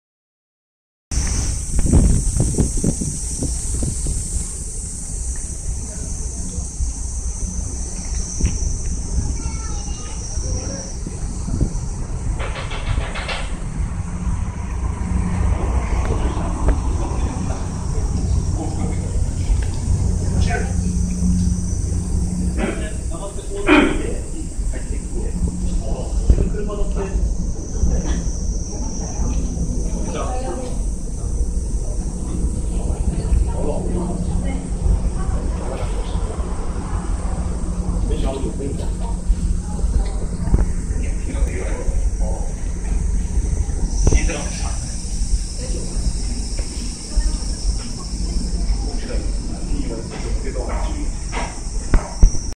北九州市門司区 関門トンネル人道入り口
説明　　外壁工事中です(笑)